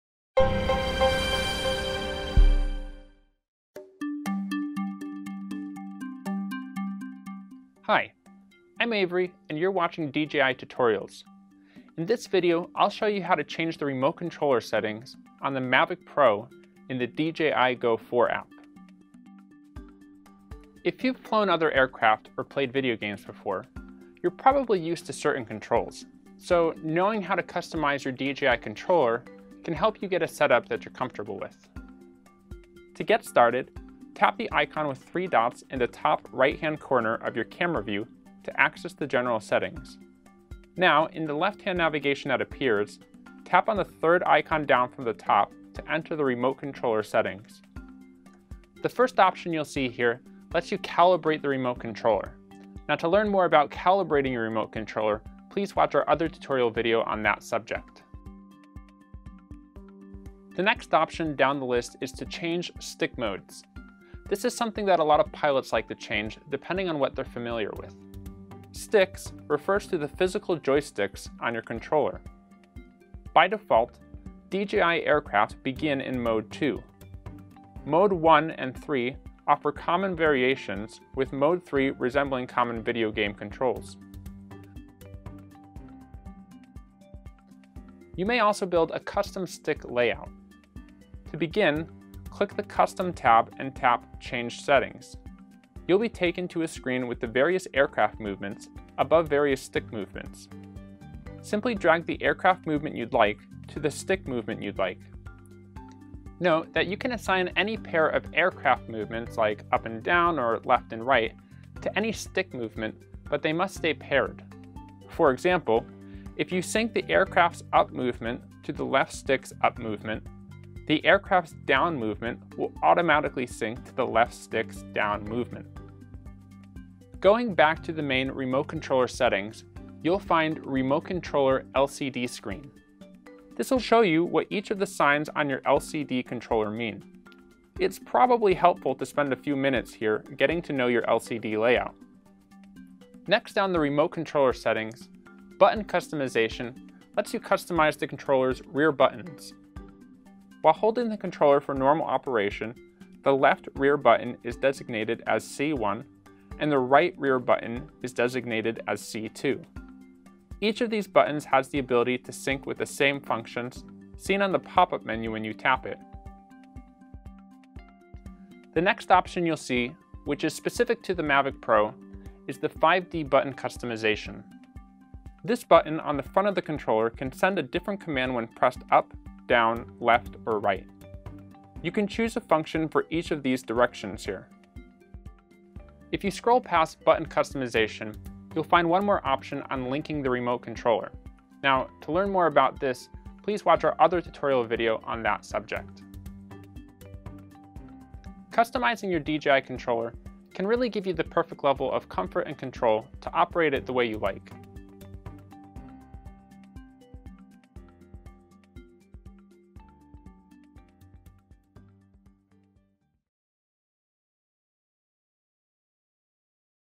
DJI Tutorials - Mavic Pro - Controller Settings
Learn how to adjust the settings on the Mavic Pro remote controller.